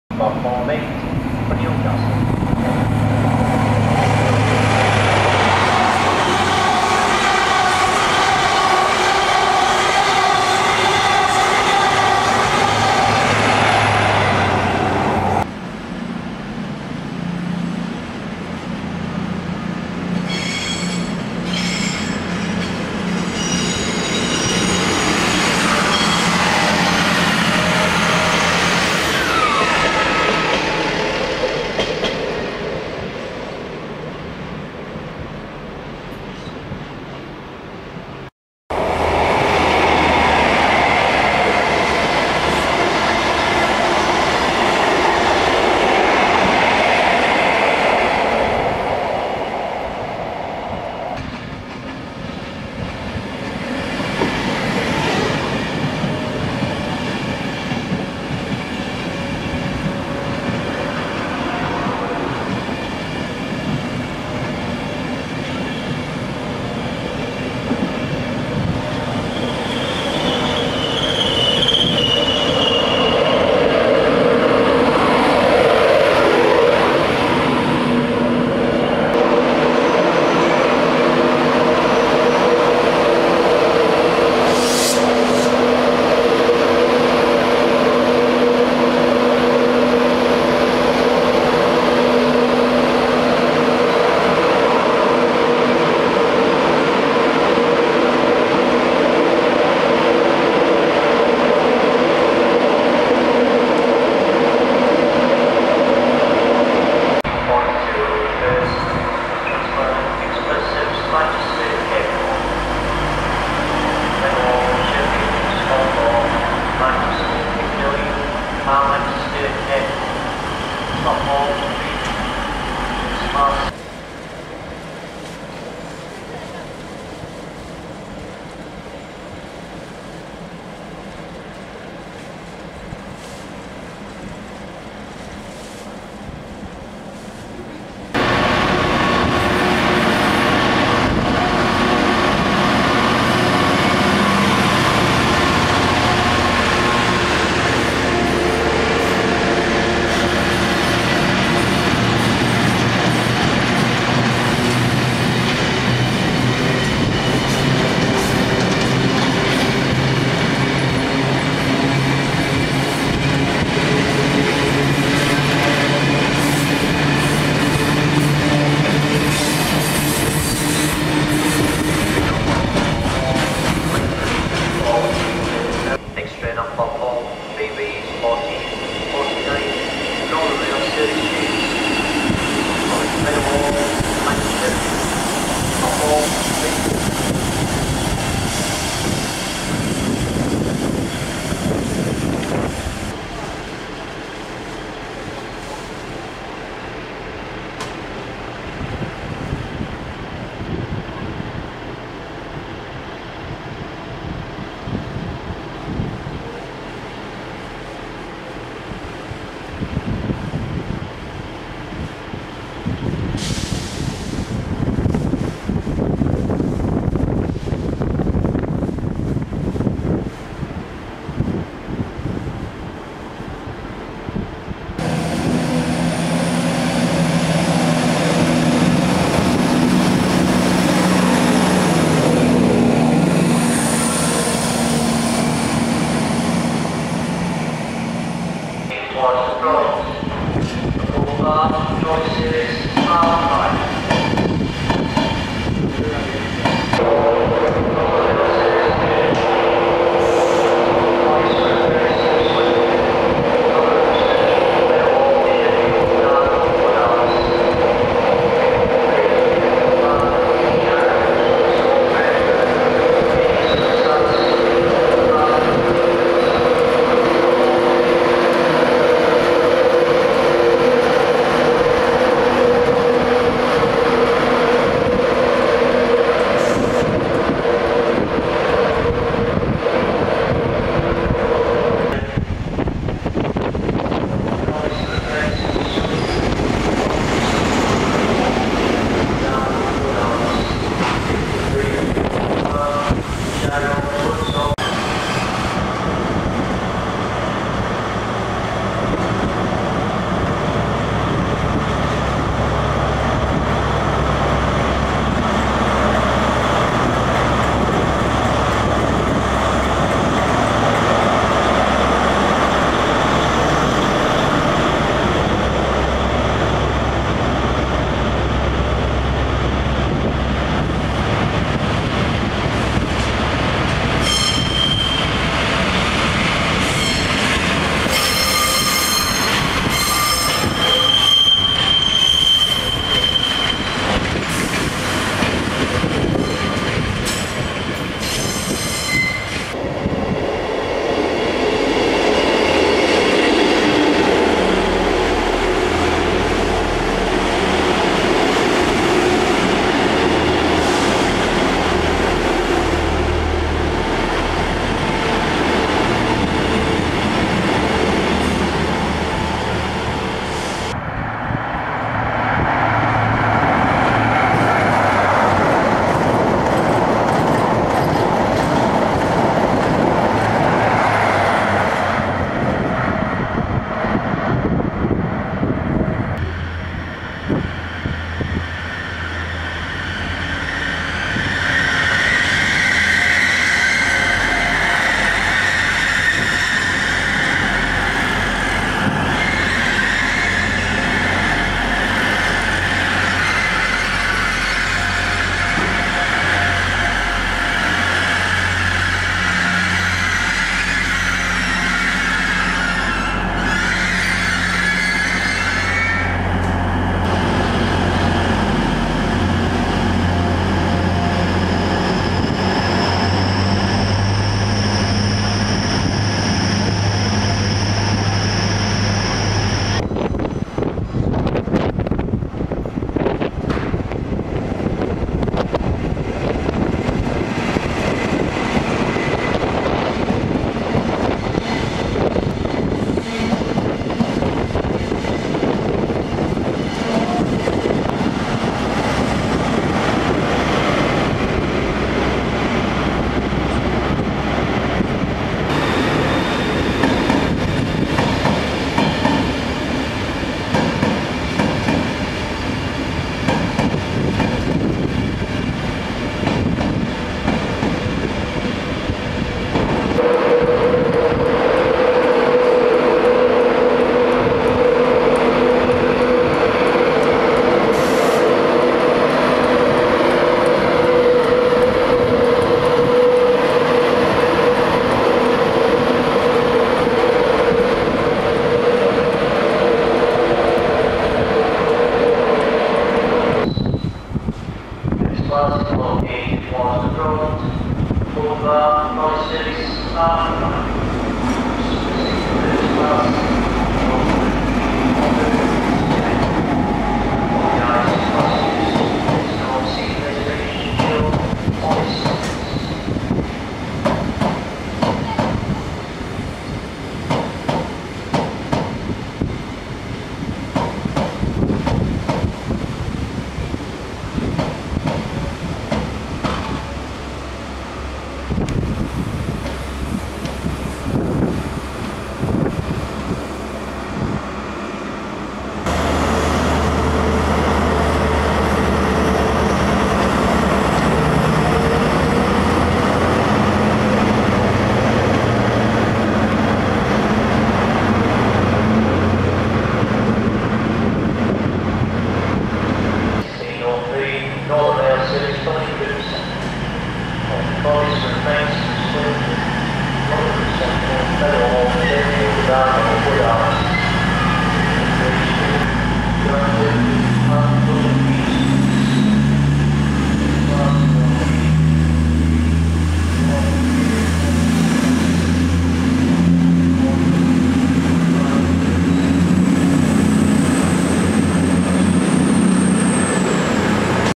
Doncaster Station August 2015
A visit to the East Coast Mainline where we see an interesting mix of colours on the Virgin East Coast Services before they are all standard red and white by the end of the year. I love the HST which is CrossCountry on one end, Virgin on the other and National Express in between! It will be relatively boring when they are all the same in the near future. Some interesting trains in the yard opposite; some old Gatwick Express Trains I do believe.Other services are made up of the usual East Coast mix: Transpennine Express, Northern, CrossCountry, Hull Trains and Grand Central.